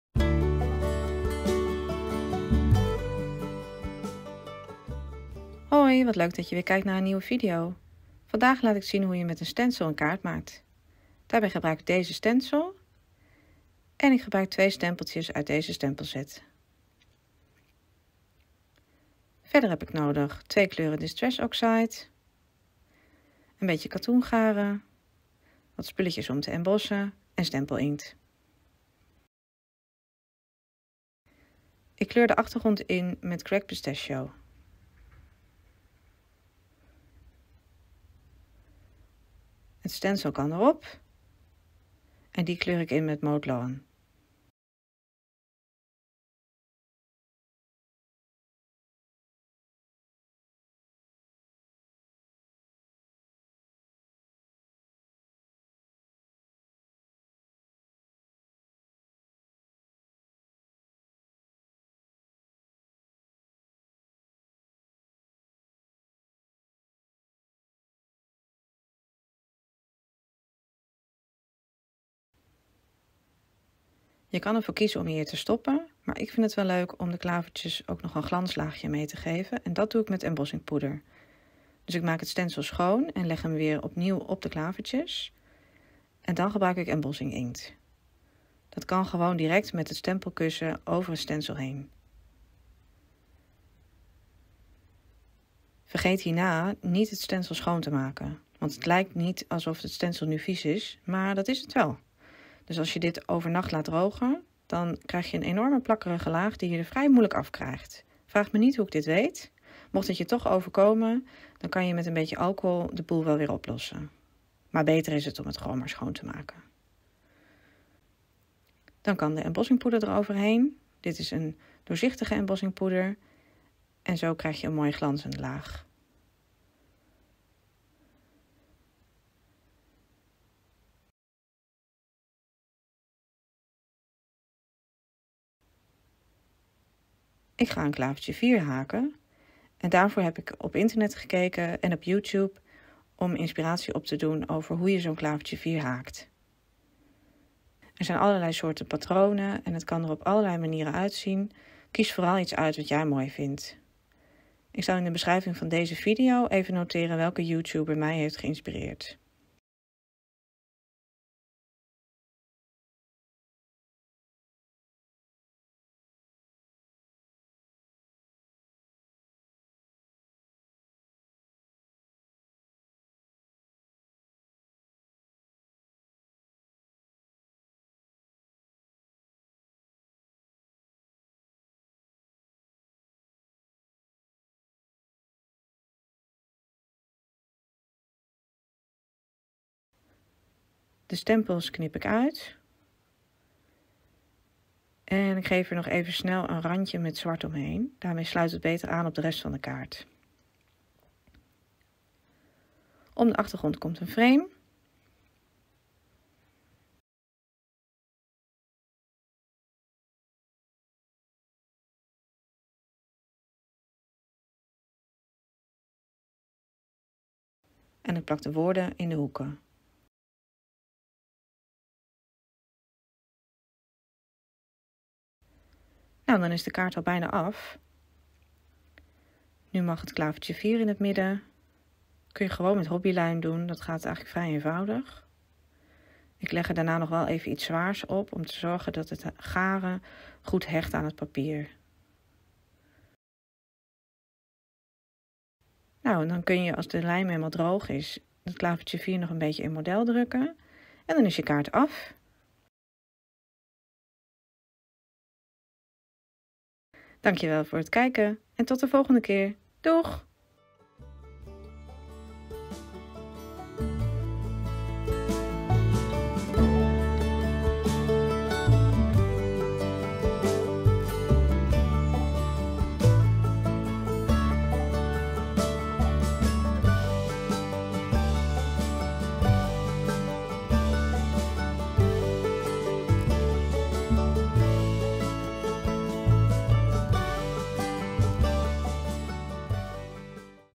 Een kaartje brengt geluk!
Een mixed media kaartje met een klavertje vier dat ik zelf heb gehaakt, weer eens wat anders! Kijk met me mee hoe ik met een stencil, inkt en wat garen een gezellig kaartje knutsel.

Werkwijze klavertje vier (Youtube video):
How to Crochet a Four Leaf Clover - B.Hooked Crochet (ik heb zelf nog wel het steeltje verlengd met chain 4).